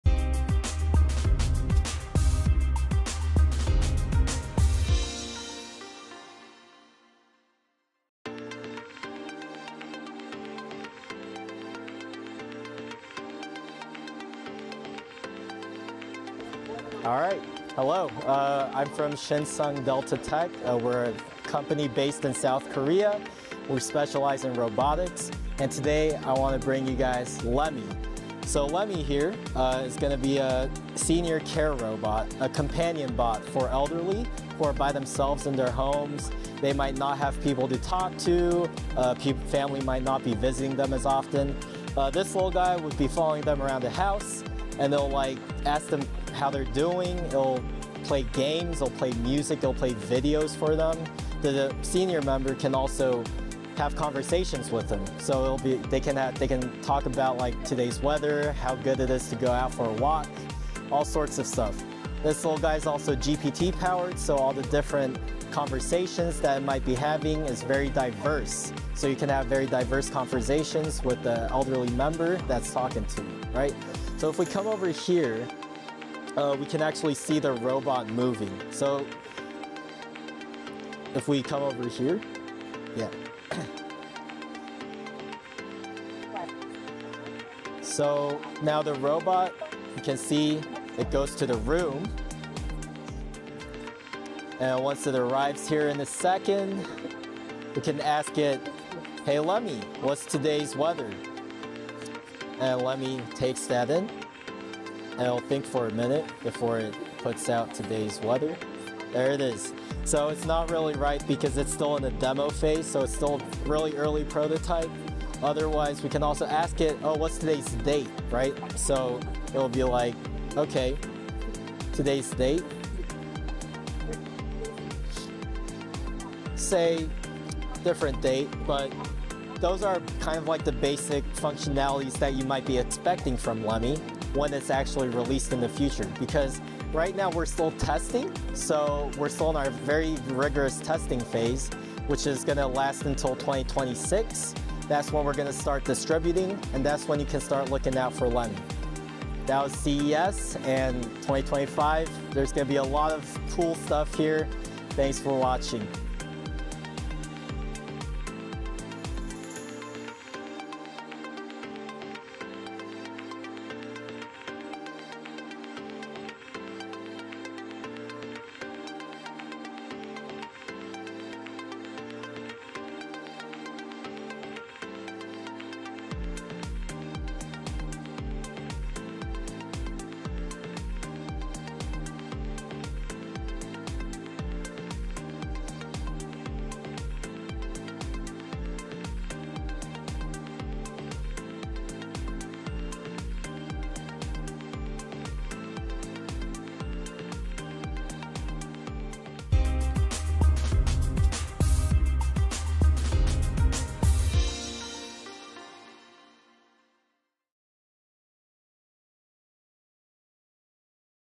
Lemmy the Robot! The Future of AI & Robotics 🤖
We just explored Lemmy, the cutting-edge robot that’s redefining what AI and automation can do! 🔥

✅ What makes Lemmy unique?
✅ How does it interact with humans?
✅ The future of robotics and AI!

Check out the full video and see Lemmy in action!